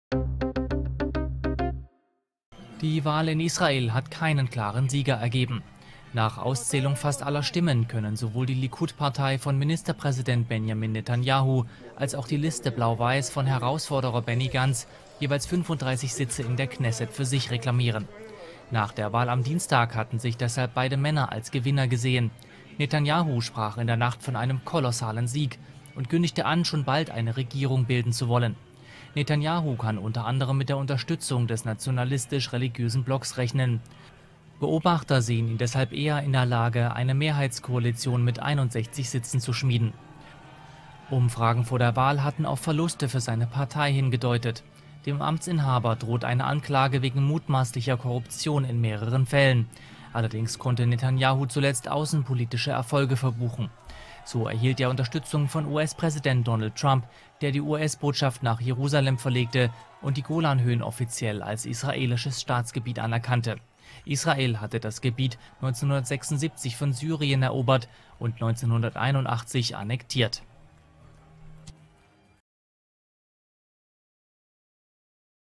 Kein klarer Sieger nach Wahl in Israel
Benjamin Netanjahu und Benny Gantz eroberten mit ihren Parteien jeweils 35 Sitze in der Knesset.